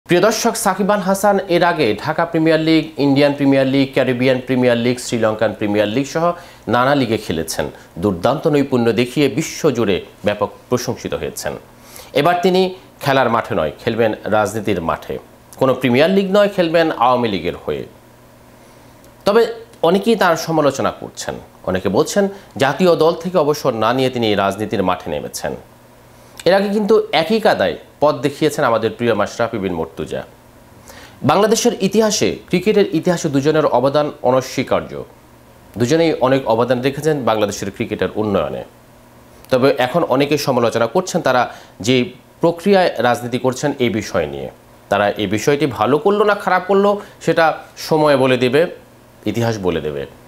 সাকিব; প্রিমিয়ার লীগ থেকে আওয়ামী লীগে।
ক্রিকেট লীগ থেকে আওয়ামী লীগে সাকিব আল হাসান;